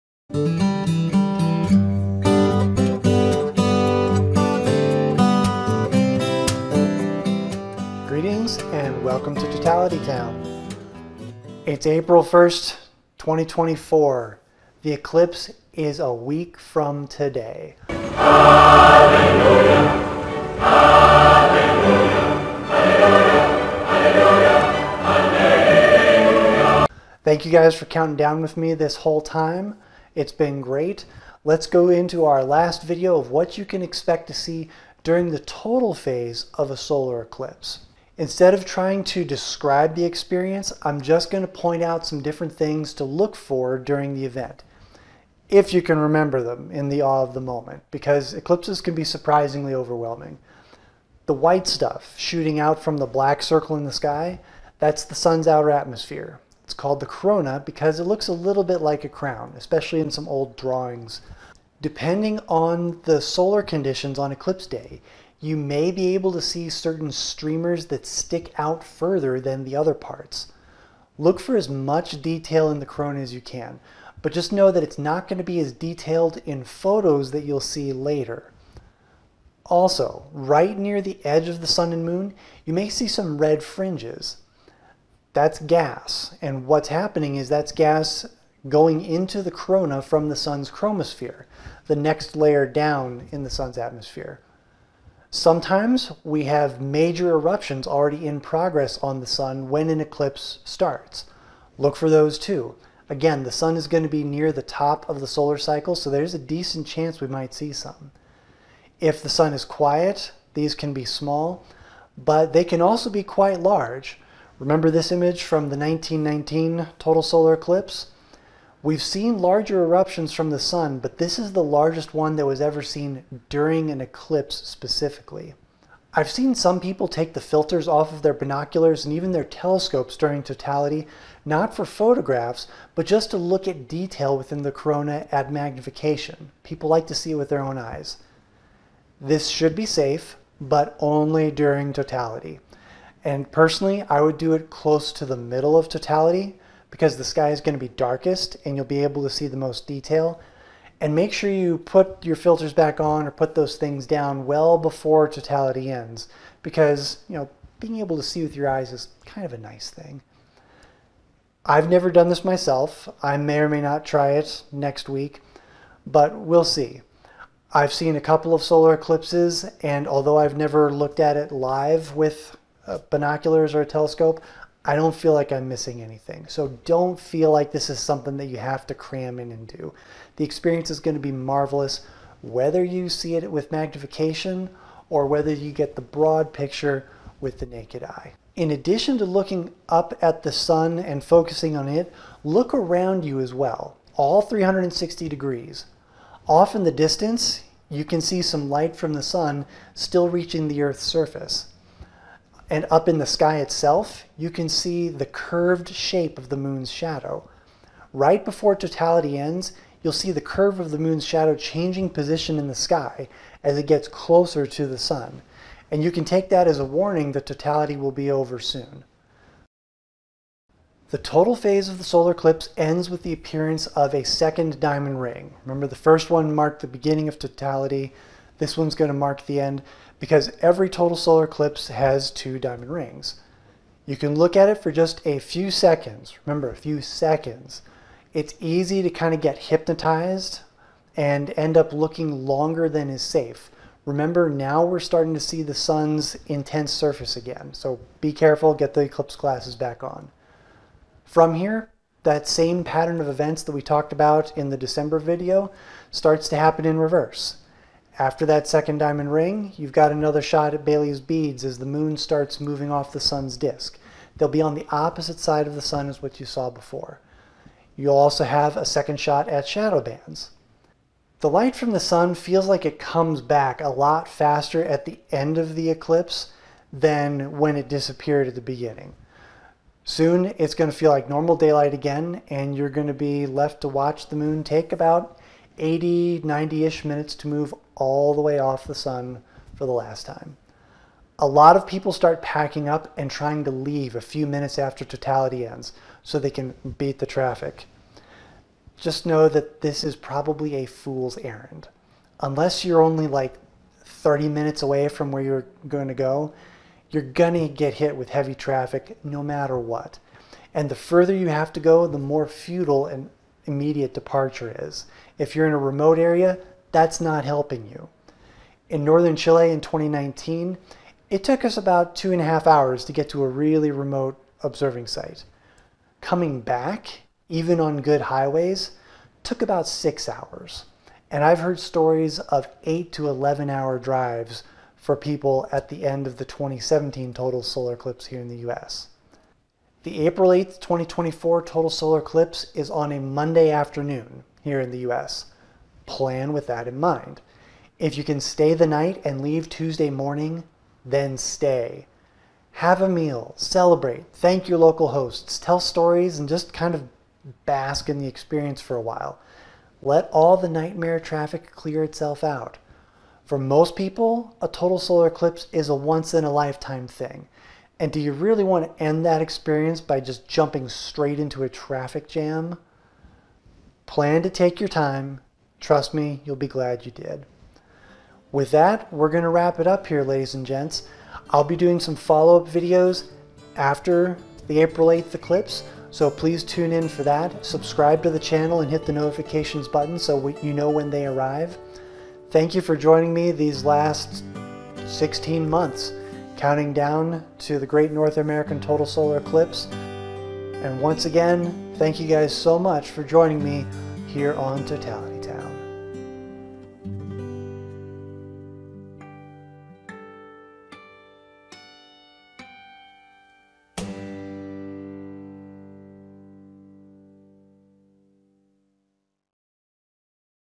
April 2024 - What to Look For During Totality
Part 1 (back in Dec 2023) was about what to look for leading up to totality.  Now time for the main event!!  What should you be looking for as the skies grow dark?